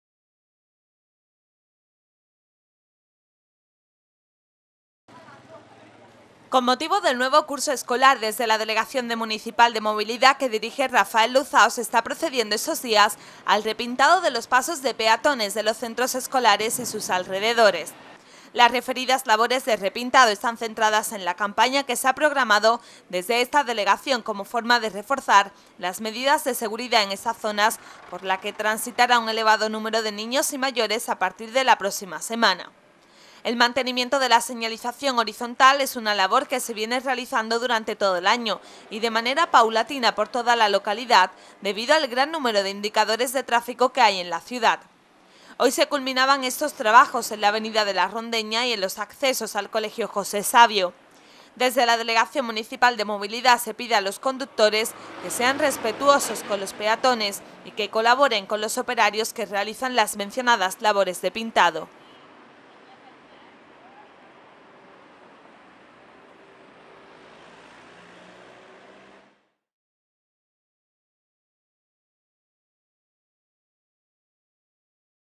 Trabajos de repintado de pasos de peatones por la ciudad, SanlúcarNoticias 2014
La delegación de movilidad se dedica durante estos días al repintado de los pasos de peatones de la ciudad.
Una labor que se desarrolla durante todo el año, y que ahora se centro en zonas de centros escolares y alrededores.